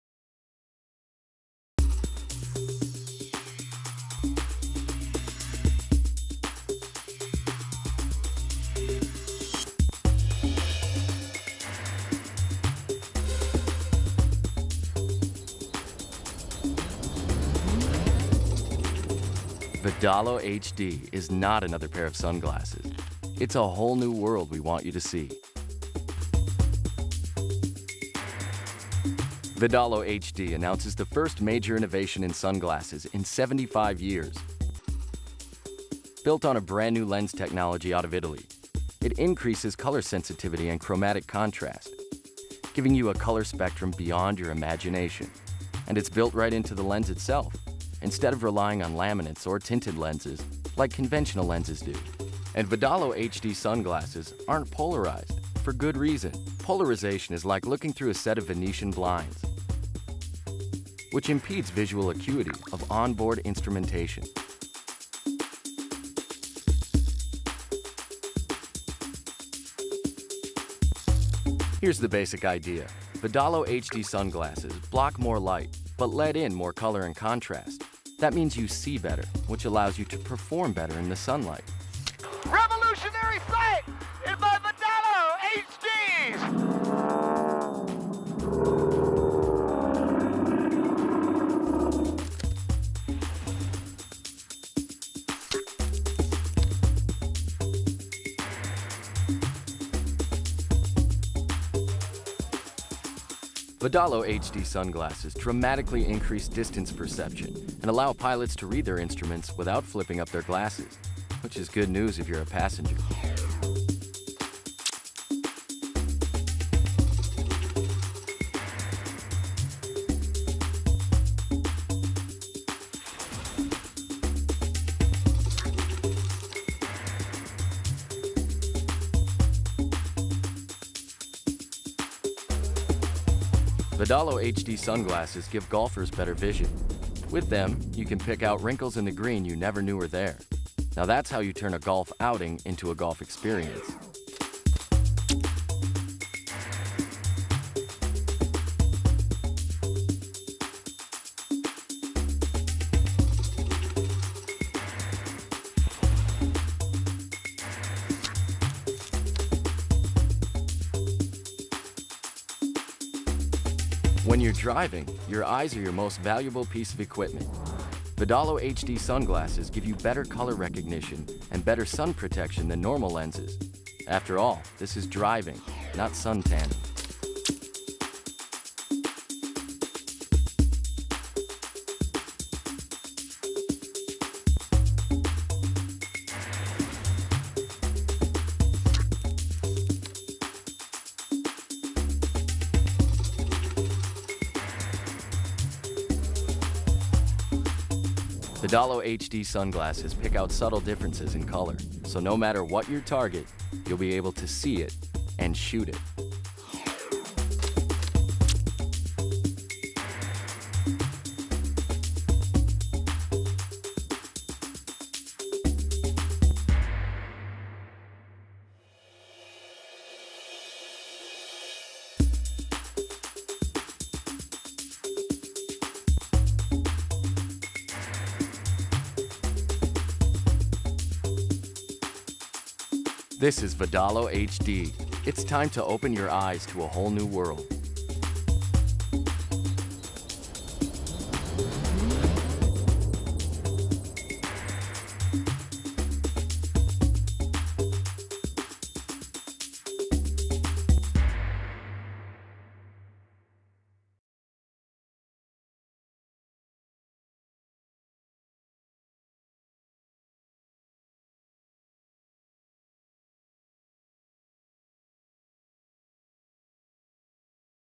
VedaloHD Performance Sunglasses Informational Script
See how VedaloHD Performance Sunwear uses propreitary sun filtering science to improve your visual performance. It is truly Revolutionary Sight.